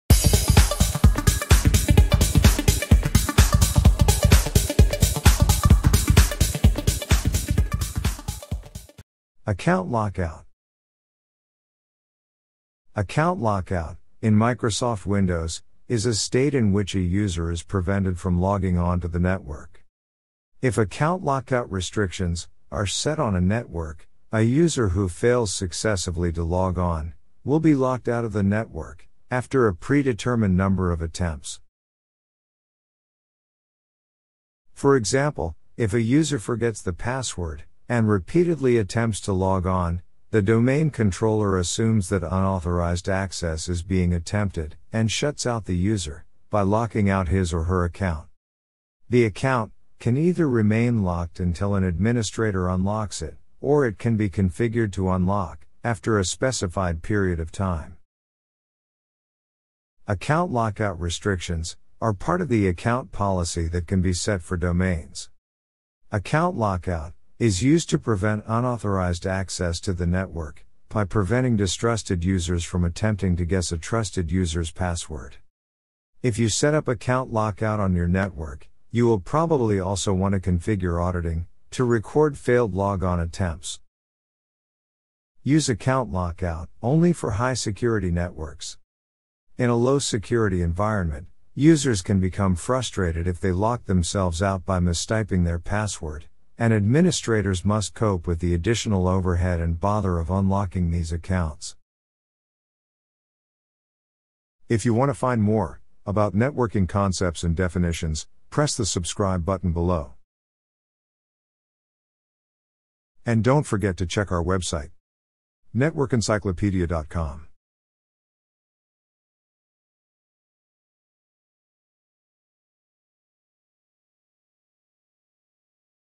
Account Lockout - Network Encyclopedia
Account Lockout, in Microsoft Windows, is a state in which a user is prevented from logging on to the network. If account lockout restrictions are set on a network, a user who fails successively to log on will be locked out of the network after a predetermined number of attempts.

Account lockout restrictions are part of the account policy that can be set for domains. Account lockout is used to prevent unauthorized access to the network by preventing distrusted users from attempting to guess a trusted user’s password.